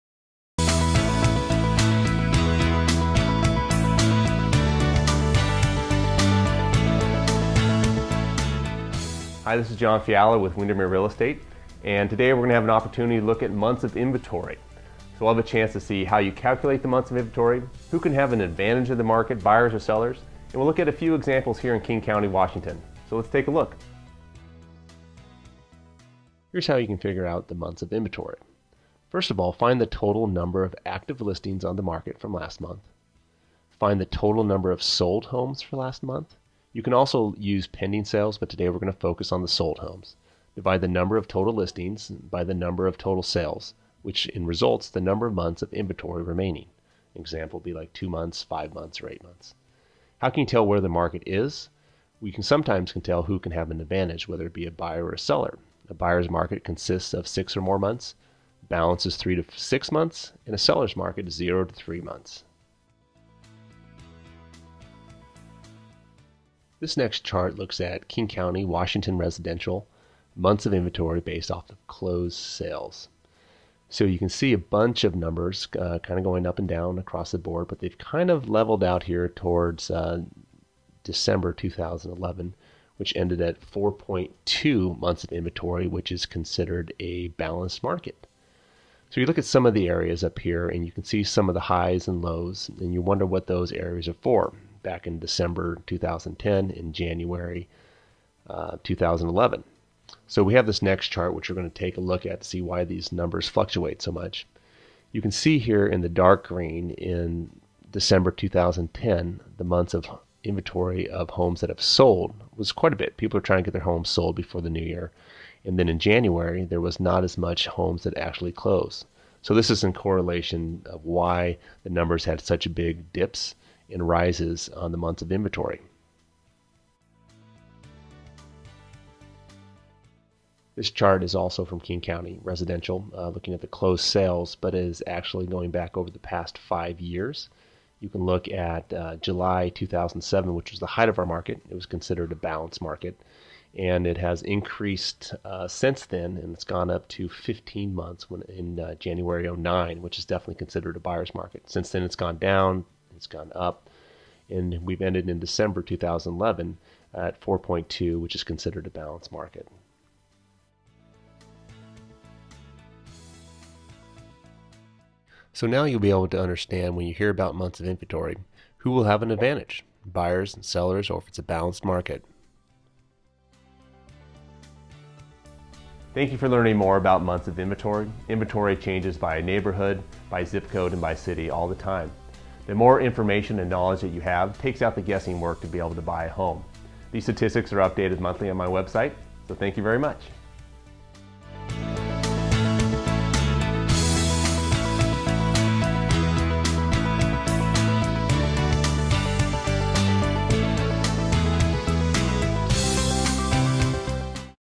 Months of Inventory Definition
This video takes a look at how to calculate Months of Inventory in a real estate market. There are advantages in the market for Buyers and Sellers depending on the Months of Inventory. The graphs in this video shows King County Washington for 2011 and then a five-year period of King County.